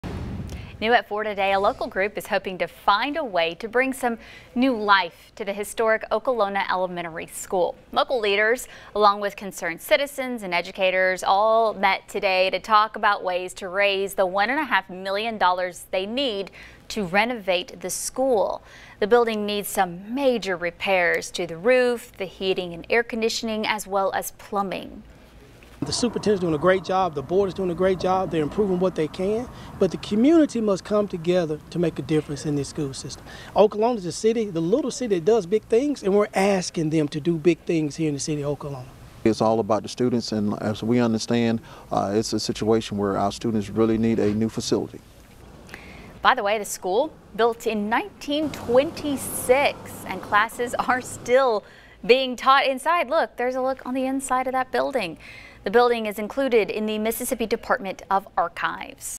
Local group seeks money to renovate Okolona Elementary School
A local group is seeking money to renovate Okolona Elementary School.